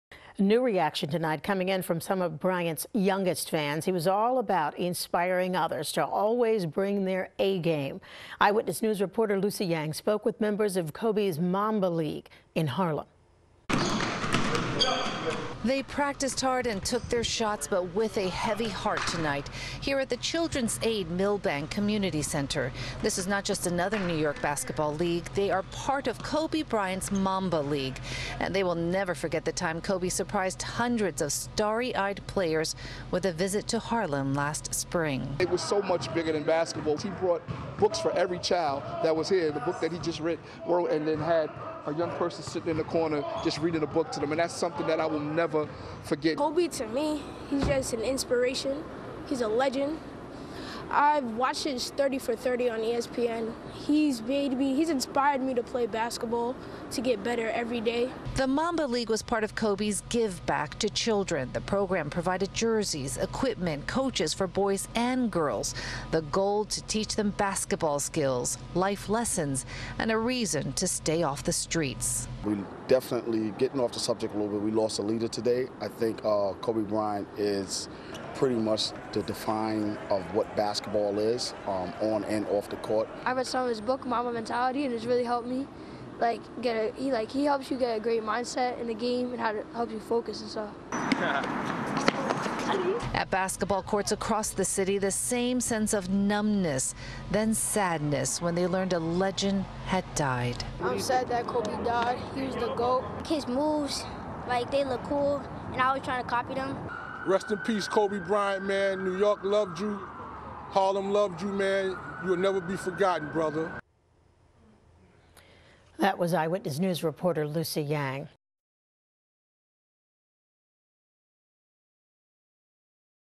Bigger than Basketball: Kobe Bryant's 'Mamba League' in Harlem
Lucy Yang reports from Harlem.